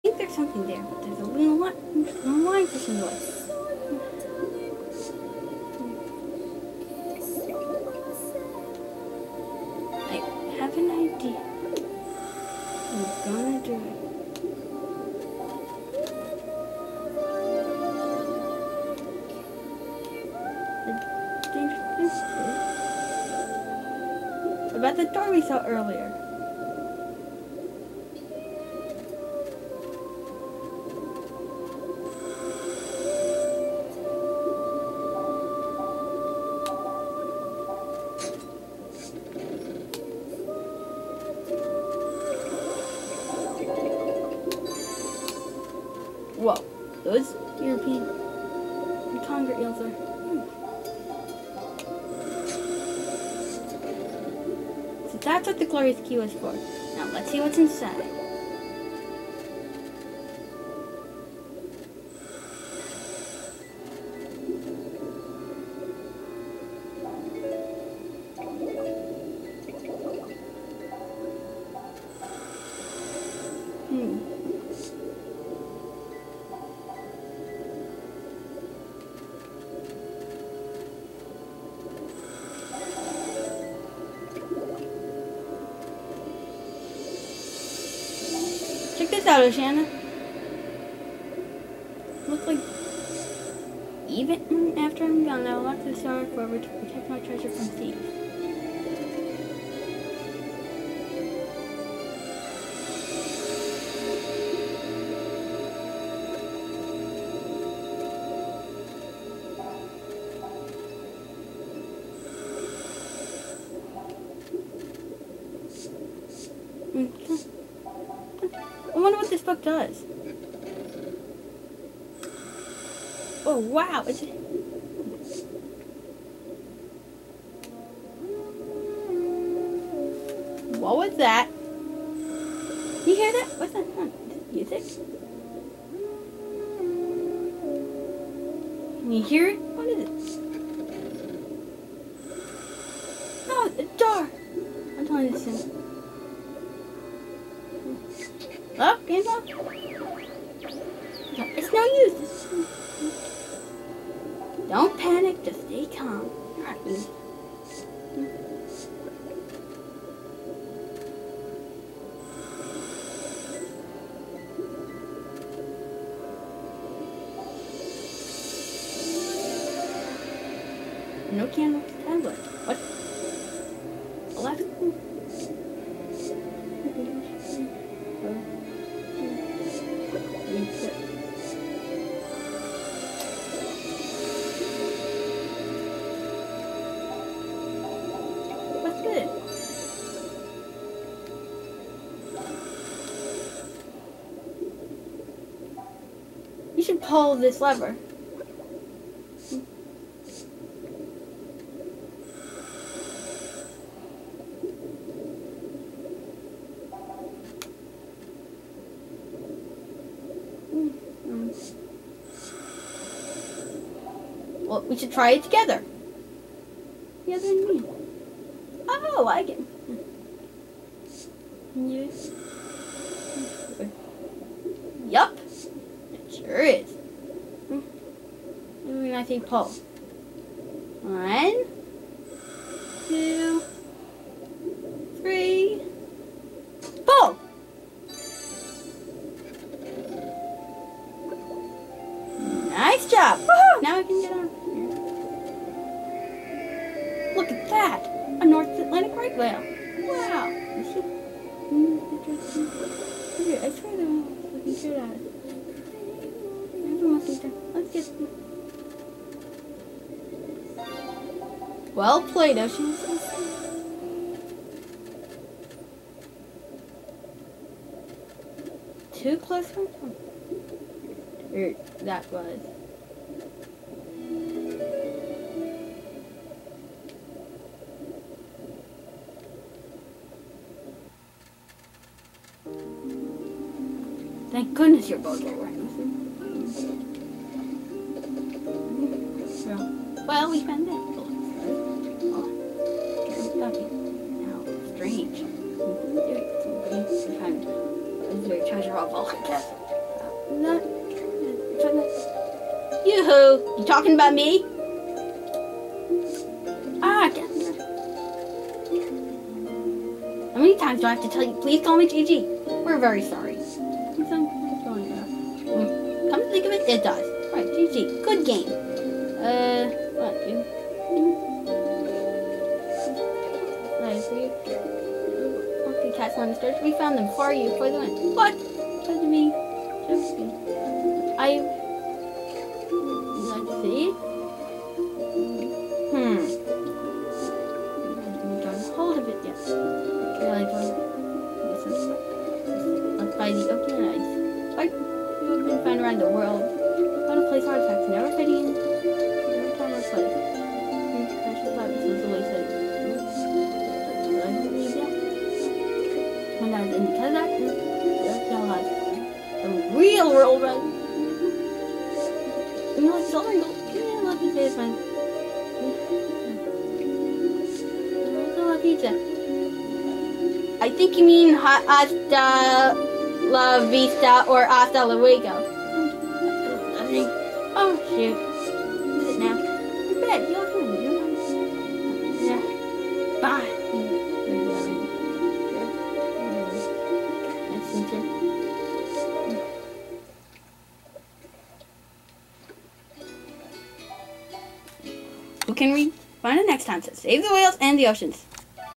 The Blue World Episode 9: All bets are Off
This episode wraps up L&L's investigation of Valka Castle. That message written in Latin on the door to the King's Chamber led us to find the Glorious Key, which was under a Luna Lionfish, The King's Chamber also held an Okeanos Tablet. For the first time, the Song of Dragons was heard. Oceana and I had to work together to free ourselves. The source of the song was a North Atlantic Right Whale, which Oceana thought was LOOKING RIGHT AT HER! Since we found Valka Castle first, we won the bet we made with GG! However, much to Oceana's disappointment, GG never scraped the boat with a toothbrush...